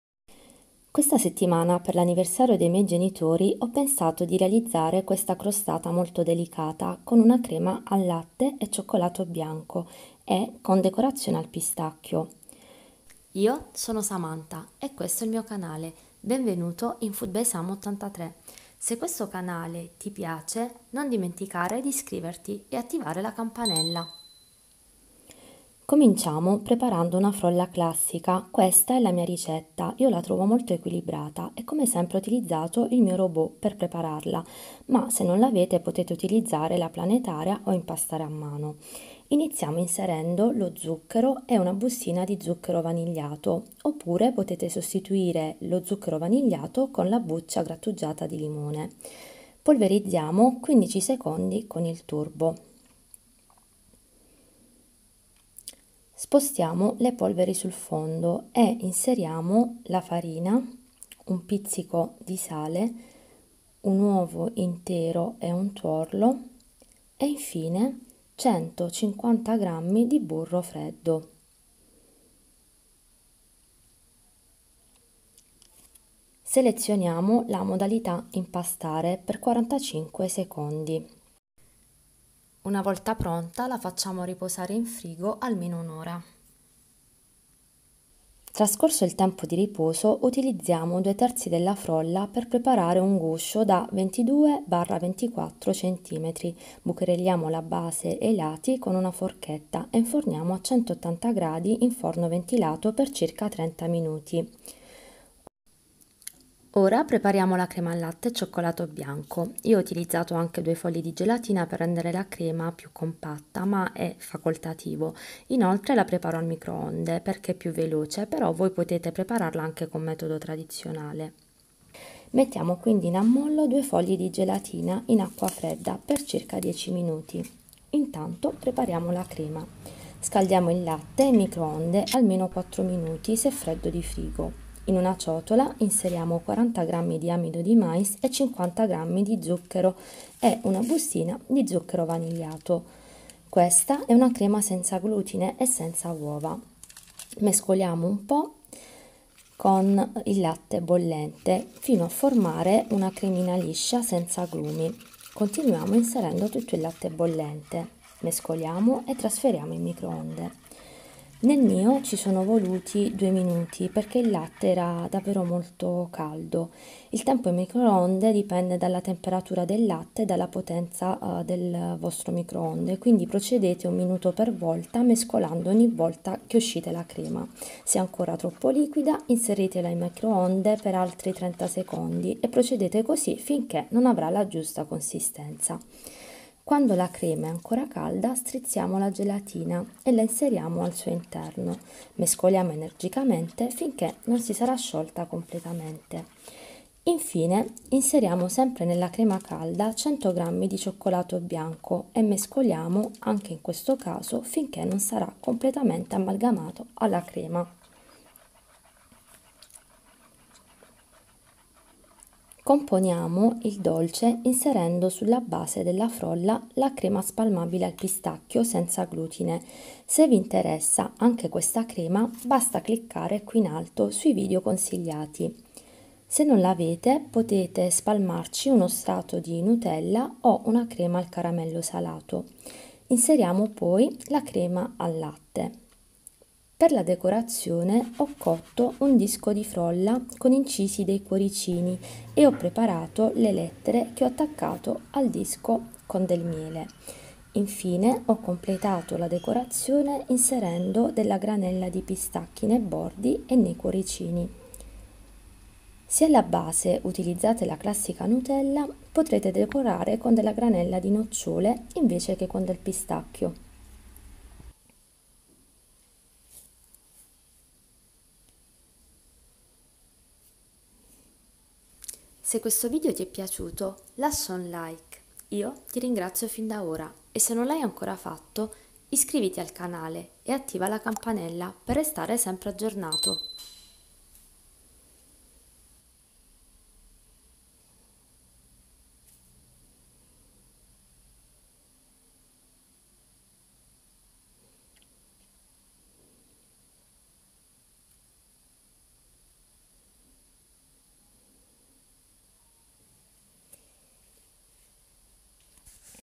Crostata con crema al cioccolato bianco e pistacchi - mounsieur cuisine connect
Questa settimana per l'anniversario di matrimonio dei miei genitori ho ideato e preparato questa crostata con crema al latte e cioccolato bianco e pistacchi. Questa ricetta è una mia creazione e quando l'ho assaggiata ne sono rimasta entusiasta.

Forse pochi sanno che molte delle mie ricette (non tutte) sono frutto della mia inventiva. Molte ricette infatti non vengono publicate perchè non soddisfano il mio palato,infatti  il mio desiderio è quello di pubblicare solo ricette collaudate che in primis piacciono a me.

Quando ho ideato questa ricetta non sapevo ancora se sarebbe stata pubblicata, ma è davvero buonissima e si è meritata la pubblicazione, spero che piacerà tanto anche a voi.

Vi lascio la lista degli ingredienti

Frolla:
120 g di zucchero
zucchero vanigliato o buccia di limone
300 g di farina
un pizzico di sale
1 uovo e 1 tuorlo
150 gr di burro freddo

Per la crema:
500 ml di latte bollente
40 gr di amido di mais
50 gr di zucchero
zucchero vanigliato
2 fogli di gelatina
100 g di cioccolato bianco

Se avete dubbi o avete bisogno di chiarimenti scrivetemi nei commenti.

Grazie per i vostri like. Se questo canale vi piace continuate a supportarmi  iscrivendovi in tanti!